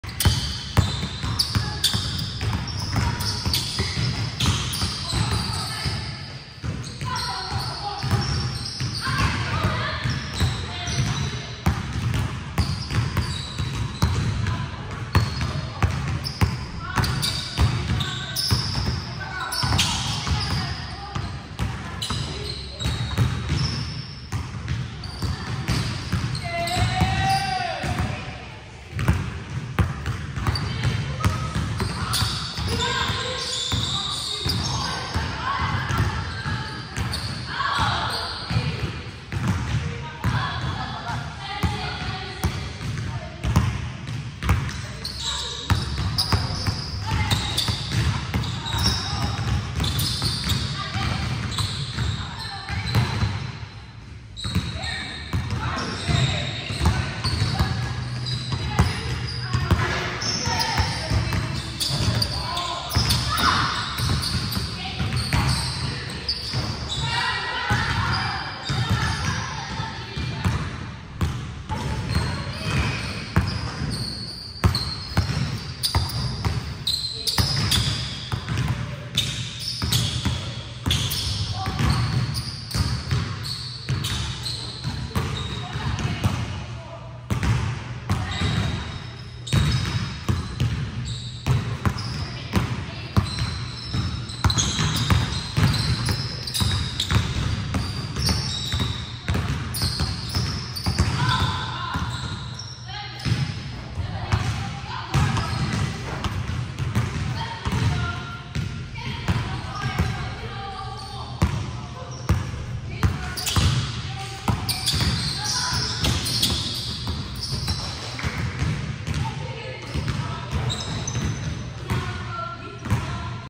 Makings moves and layup finishes
Making a few moves and finishing layups.